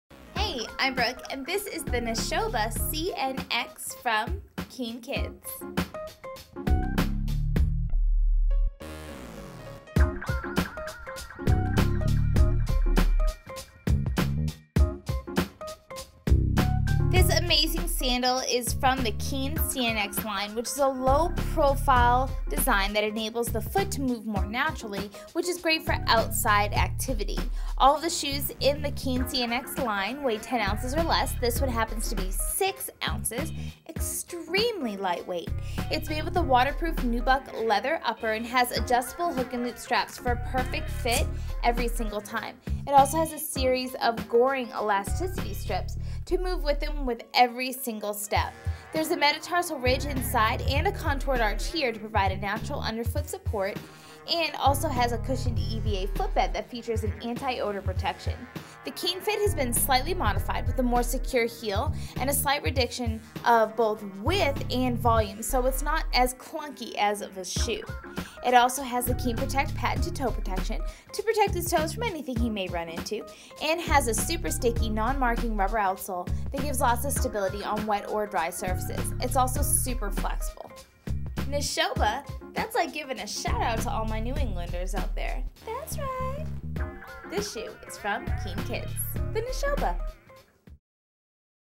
Keen kids nashoba cnx little kid big kid forest night
View the size chart

    He'll be on the move all day long with the lightweight and supportive Nashoba CNX sandal from Keen®!
    Keen CNX is low profile enabling the foot to move naturally.
    Waterproof nubuck leather upper.
    Adjustable hook-and-loop closure for easy on and off.
    Metatarsal ridge and contoured arch provide natural underfoot support.
    Cushioned EVA footbed features anti-odor protection.
    The Keen fit is slightly modified, with a more secure heel and a slight reduction of width and volume.
    KEEN.PROTECT™ patented toe protection.
    Grippy, non-marking rubber outsole ensures traction on wet or dry surfaces.
    All Keen CNX shoes and sandals weigh 10 ounces or less.
    Imported.